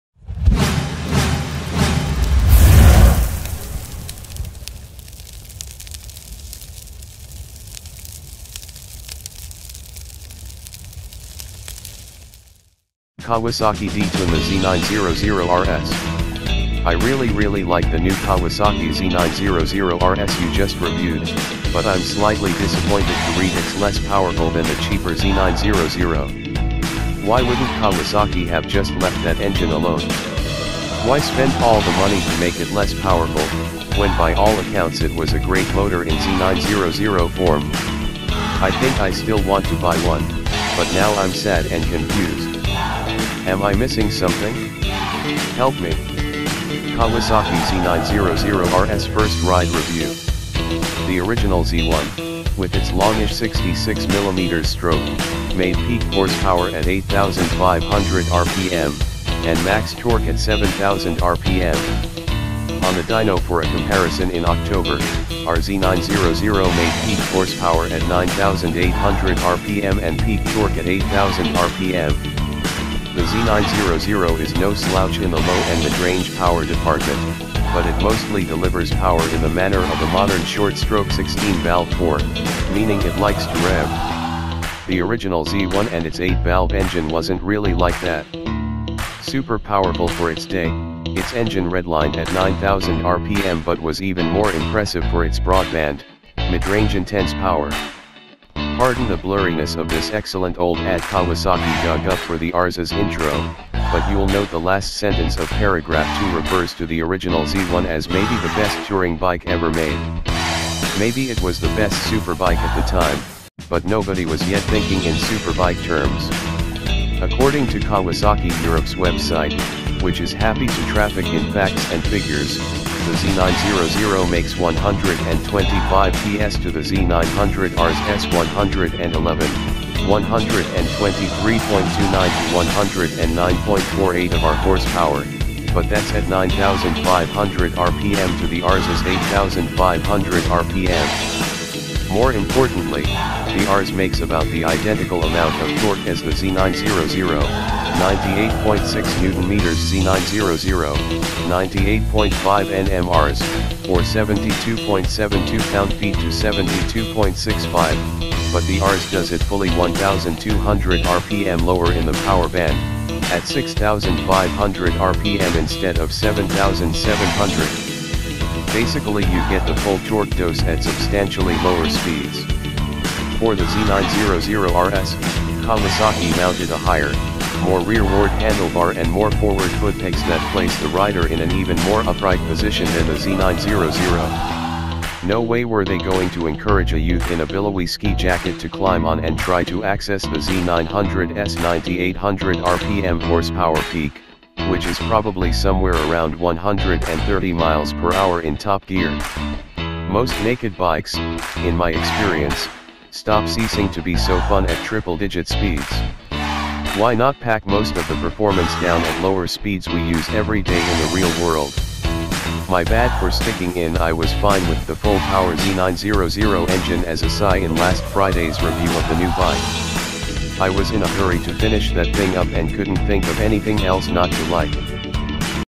Kawasaki Detune The Z900RS
I really really like the new Kawasaki Z900RS you just reviewed, but I’m slightly disappointed to read it’s less powerful than the cheaper Z900. Why wouldn’t Kawasaki have just left that engine alone? Why spend all the money to make it less powerful, when by all accounts it was a great motor in Z900 form? I think I still want to buy one, but now I’m sad and confused. Am I missing something? Help me
Kawasaki Z900RS First Ride Review
The original Z1, with its longish 66mm stroke, made peak horsepower at 8500 rpm, and max torque at 7000 rpm. On the dyno for a comparison in October, our Z900 made peak horsepower at 9800 rpm and peak torque at 8000 rpm. The Z900 is no slouch in the low- and midrange power department, but it mostly delivers power in the manner of a modern short-stroke 16-valve Four – meaning it likes to rev. The original Z1 and its 8-valve engine wasn’t really like that. Super powerful for its day, its engine redlined at 9000 rpm but was even more impressive for its broadband, midrange-intense power.
Pardon the blurriness of this excellent old ad Kawasaki dug up for the RS’s intro, but you’ll note the last sentence of paragraph two refers to the original Z1 as “maybe the best touring bike ever made.” Maybe it was the best “superbike” at the time, but nobody was yet thinking in superbike terms.
According to Kawasaki Europe’s website, which is happy to traffic in facts and figures, the Z900 makes 125 PS to the Z900RS’s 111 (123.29 to 109.48 of our horsepower), but that’s at 9500 rpm to the RS’s 8500 rpm. More importantly, the RS makes about the identical amount of torque as the Z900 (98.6 Newton-meters Z900; 98.5 Nm RS, or 72.72 pound-feet to 72.65), but the RS does it fully 1200 rpm lower in the powerband – at 6500 rpm instead of 7700. Basically you get the full torque dose at substantially lower speeds
For the Z900RS, Kawasaki mounted a higher, more rearward handlebar and more forward footpegs that place the rider in an even more upright position than the Z900. No way were they going to encourage a youth in a billowy ski jacket to climb on and try to access the Z900’s 9800-rpm horsepower peak, which is probably somewhere around 130 mph in top gear. Most naked bikes, in my experience, stop ceasing to be so fun at triple-digit speeds. Why not pack most of the performance down at lower speeds we use every day in the real world?
My bad for sticking in “I was fine with the full-power Z900 engine” as a Sigh” in last Friday’s review of the new bike. I was in a hurry to finish that thing up and couldn’t think of anything else not to like!